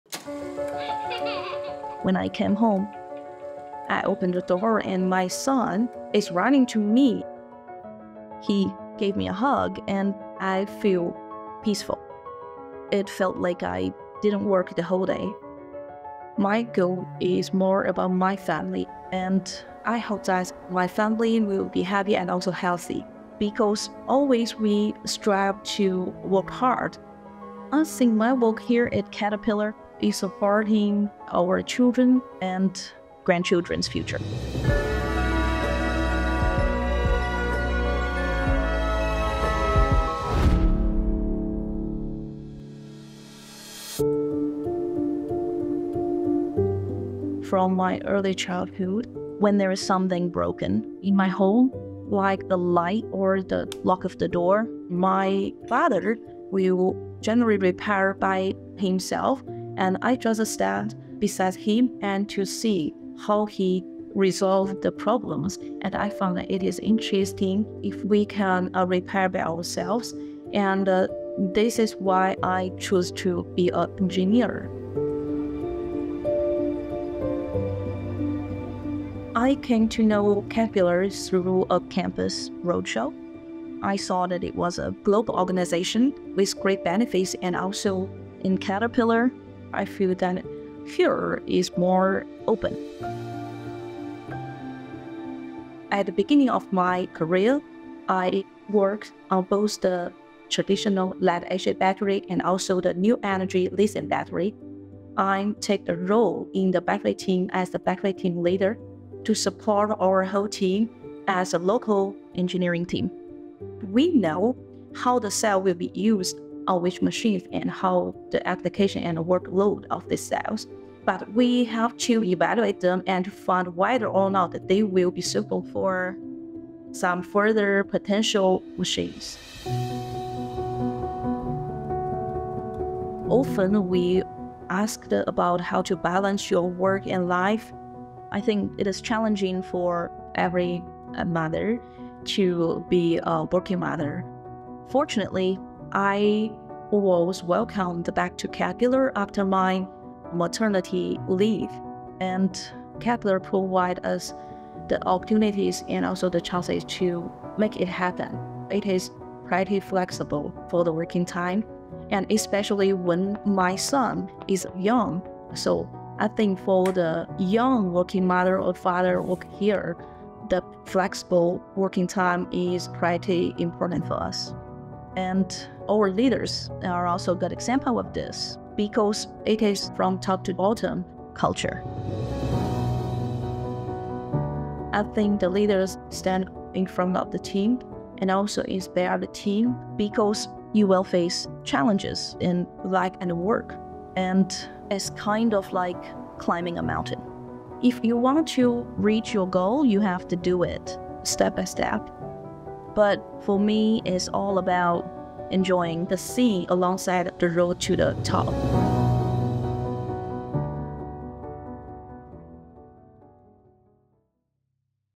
Making the World a Better Place for the Next Generation
“I hope that my family will be happy and healthy because we always strive to work hard. I see my work here, at Caterpillar, is supporting our children and grandchildren's future.” - Andy

Andy is an engineer at Caterpillar leading innovations in electrification. She aims to build a better, more sustainable world – both at work and at home - by using the flexibility her role provides to enjoy time with her family while balancing her professional achievements. Listen to her advice on excelling in all aspects of life through her practical and balanced approach as a working mother.